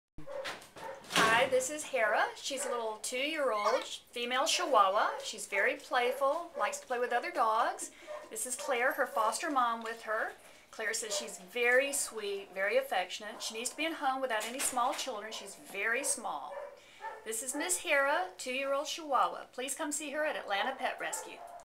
Hera: adopt me!
Hera is a two and a half year old purse-sized Chihuahua who weighs just 5 pounds. She has the tiniest body and legs, ever!  This little cutie was surrendered by her former owner because they could no longer care for her. Hera has a scar down her back that, according to her former owner, was from a coyote attack. We are not sure how she could have survived something like that, but it does not slow her down one bit.

Hera is timid at first in her new surroundings and will take a minute or two to adjust. She is just too cute for words, though her picture does not do her justice.  Hera promises to make a wonderful lifetime companion, but because of her tiny size, would do best in a home without young children.